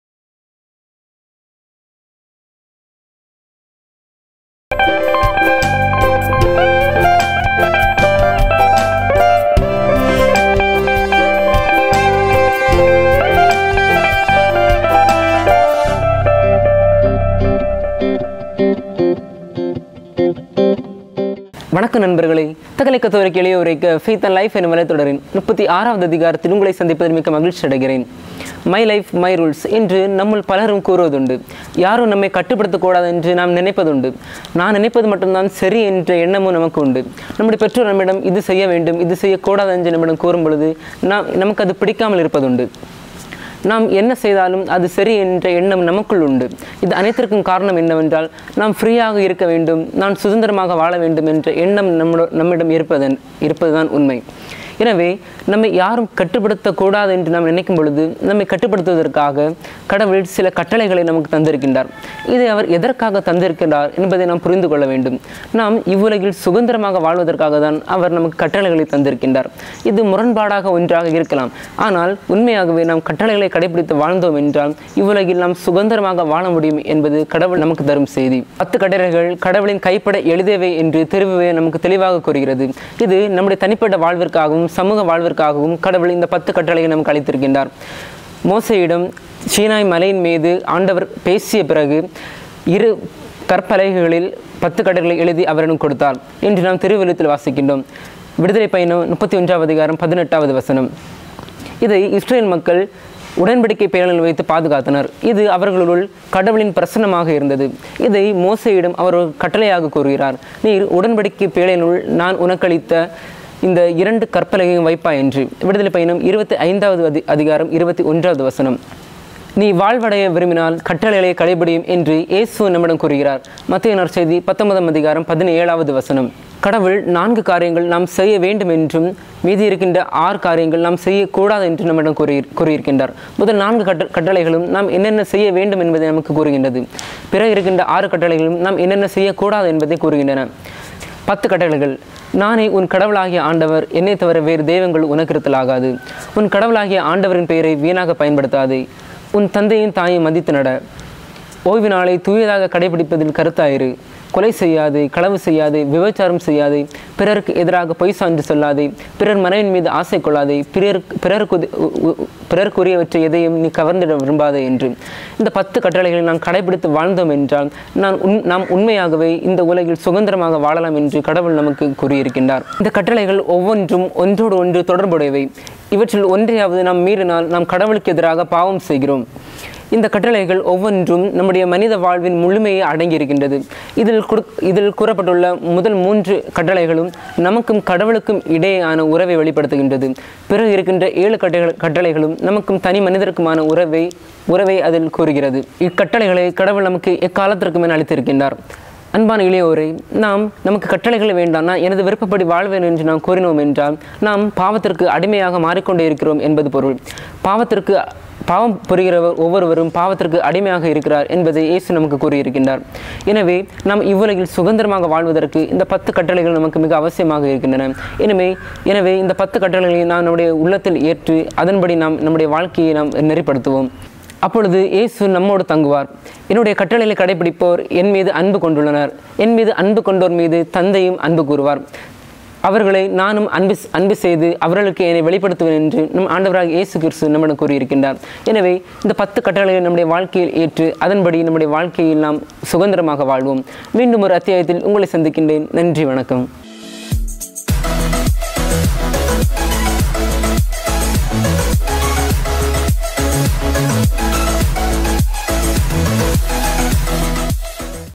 Faith & Life 36 | YOUCAT | கடவுளின் கட்டளை | அத்தியாயம் 36
Web series on Faith & Life which teaches the value of life. It's a faith journey through the Youth Catechism books YOUCAT and DOCAT
Direction : frSanthosh​​​​
Cast: Merbin Dhas

*Youcat - Episode 36 -  கடவுளின் கட்டளைகள்*

My Life My Rules என்று இளையோர் பலர் சொல்வதுண்டு. யாரும் நம்மைக் கட்டுப்படுத்தக் கூடாது என்று நினைப்போம். நாம் நினைப்பதே சரி என்ற எண்ணமும் நம்முள் பலருக்கு உண்டு. பெற்றோர் சில காரியங்கள் செய்யக் கூடாது என்று சொல்வது கூட நமக்குப் பிடிப்பதில்லை. இதுவே சுதந்திரம் என்று நினைப்பதுண்டு. Freeya இருக்கணும் என்ற எண்ணம்.
இப்படி யாரும் நம்மை கட்டுப்படுத்தக் கூடாது என்று நினைக்கும் போது, கடவுள் நம்மைக் கட்டுப்படுத்துவதற்காக சில கட்டளைகளைத் தந்திருக்கிறார். இவற்றை எதற்காக தந்திருக்கிறார் என்றால் நாம் சுதந்திரமாக இருப்பதற்காக. கட்டளைகளைத் தந்து அவற்றைக் கடைப்பிடித்தால் சுதந்திரமாக இருப்போம் என்கிறார் கடவுள். இது ஒரு முரண்பாடாக தோன்றலாம். கட்டுப்பாட்டுடன் வாழ்ந்தால் சுதந்திரமாக வாழலாம் என்பது கடவுள் தரும் பெரிய செய்தி.
பத்துக்கட்டளைகளை கடவுள் தம் கைப்பட எழதியவை என்று விவிலியம் கூறுகிறது. தனிமனிதரின் நல்வாழ்விற்காகவும் சமூக நலனுக்காகவும் பத்துக்கட்டளைகளை அளித்தார்.
“ஆண்டவர் சீனாய்மலைமேல் மோசேயோடு பேசி முடித்தபின், கடவுளின் விரலால் எழுதப்பட்ட கற்பலகைகளான உடன்படிக்கைப் பலகைகள் இரண்டையும் அவரிடம் அளித்தார்” (விப 31:18).
இந்தப் பத்துக் கட்டளைகளையும் இஸ்ரயேல் மக்கள் உடன்படிக்கைப் பேழையில் வைத்து பாதுகாத்தனர். மக்கள் மத்தியில் கடவுளின் பிரசன்னமாக அவை இருந்தன. இதைக் கட்டளையாக கடவுள் மோசேயிடம் கூறினார்: “பேழையினுள் நான் உனக்களிக்கும் உடன்படிக்கைக் கற்பலகைகளை வைப்பாய்” (விப 25:21).
இக்கட்டளைகளைக் கடைப்பிடித்தால் நிலைவாழ்வு அடைய முடியும் என்று இயேசுவும் கற்பித்தார். “நீர் வாழ்வடைய விரும்பினால் கட்டளைகளைக் கடைப்பிடியும்” (மத் 19:17).
கடவுள் நான்கு காரியங்கள் செய்ய வேண்டும் என்றும் 6 காரியங்கள் செய்யக்கூடாது என்றும் கூறியிருக்கிறார்.  பத்துக்கட்டளைகளில் முதலில் வரும் நான்கும் என்னென்ன செய்ய வேண்டும் என்று கூறுகிறது, மற்ற ஆறும் எதை எதைச் செய்யக்கூடாது என்று கூறுகிறது.
பத்துக் கட்டளைகள்
1. நாமே உன் கடவுளாகிய ஆண்டவர். எம்மைத் தவிர வேறு தெய்வங்கள் உனக்கு இருத்தல் ஆகாது.
2. உன் கடவுளாகிய ஆன்டவரின் பெயரை வீணாகப் பயன்படுத்தாதே.
3. ஓய்வுநாளைத் தூயதாகக் கடைப்பிடிப்பதில் கருத்தாய் இரு.
4. உன் தந்தையையும் தாயையும் மதித்து நட.
5. கொலை செய்யாதே.
6. விபச்சாரம் செய்யாதே.
7. களவு செய்யாதே.
8. பிறருக்கு எதிராகப் பொய்ச் சான்று சொல்லாதே.
9. பிறர் மனைவிமீது ஆசை கொள்ளாதே.
10. பிறருக்கு உரியது எதையும் கவர்ந்திட விரும்பாதே
இந்தப் பத்துக்கட்டளைகளையும் கடைப்பிடித்தால் நாம் சுதந்திரமாக வாழலாம். இவை ஒவ்வொன்றும் ஒன்றோடொன்று தொடர்புடையவை. ஒருக் கட்டளையை புறக்கணிப்பவரும், அதை மீறுபவரும் கடவுளுக்கு எதிராக பாவம் செய்கிறார்.
இக்கட்டளைகளுக்குள் மனித வாழ்வின் முழுமையும் அடங்கியுள்ளது. இதில் முதல் மீன்று கட்டளைகளும் கடவுளுக்கும் தனிமனிதருக்குமான உறவு சம்மந்தமானது. பிற 7 கட்டளைகளும் பிற மனிதரோடான உறவைச் சார்ந்தவை.
இக்கட்டளைகளைக் கடவுள் எக்காலத்துக்குமென தந்திருக்கிறார்.
அன்பான இளையோரே, நாம் இவற்றை மீறும் போது, பாவம் நம்மை அடிமைப்படுத்துகிறது. கட்டளை வேண்டாம் எனது விருப்பப்படி வாழ்வேன் என்று நான் கூறும் போது நான் பாவத்திற்கு அடிமையாகிறேன் என்று பொருள்.
“பாவம் செய்யும் எவரும் பாவத்திற்கு அடிமை என உறுதியாக உங்களுக்குச் சொல்கிறேன்” (யோவா 8:34) என்று இயேசு கூறுகிறார்.
பாவம் விட்டொழித்து சுதந்திரமாக வாழ பத்துக்கட்டளைகள் அவசியம். அவற்றைக் கடைப்பிடித்து சுதந்திரமாக வாழ்வோம். அப்போது இயேசு நம்மோடு தங்குவார்.
“என் கட்டளைகளை ஏற்றுக் கடைப்பிடிப்பவர் என்மீது அன்பு கொண்டுள்ளார். என்மீது அன்பு கொள்பவர்மீது தந்தையும் அன்பு கொள்வார். நானும் அவர் மீது அன்பு கொண்டு அவருக்கு என்னை வெளிப்படுத்துவேன்” (யோவா 14:21).